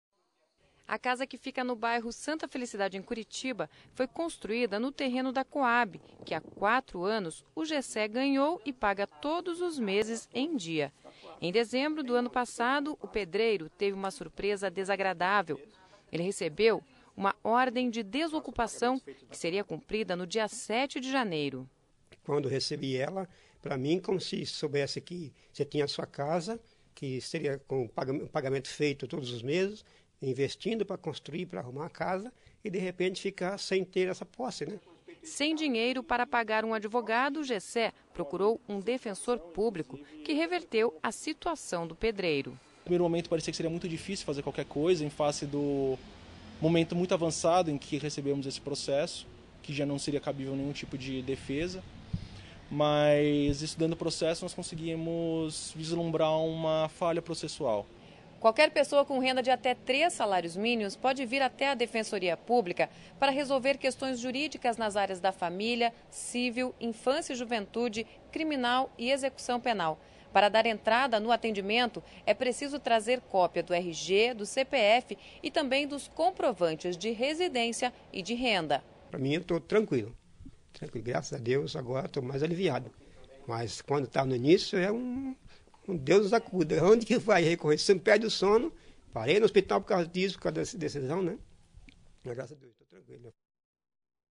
Defensoria Pública do Paraná realiza em média 650 atendimentos por mês
No ano passado, a Defensoria Pública do Paraná realizou uma média de seiscentos e cinquenta atendimentos por mês. Na instituição, são resolvidos processos judiciais de pessoas que não podem pagar um advogado.